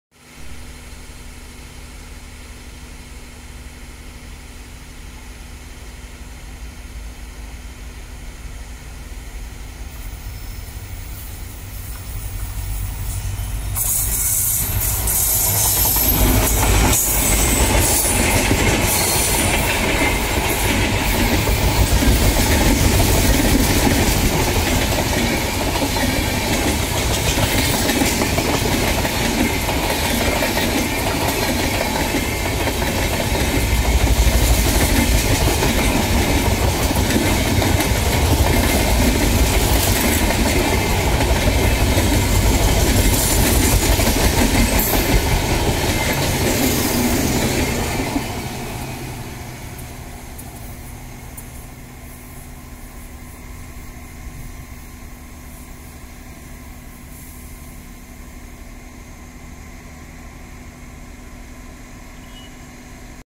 11018 karaikal- Lokmanya Tilak express heading towards Gulbarga WITH Kalyan WDP4D GAURAV
Train was in good speed 🔥 enjoy the video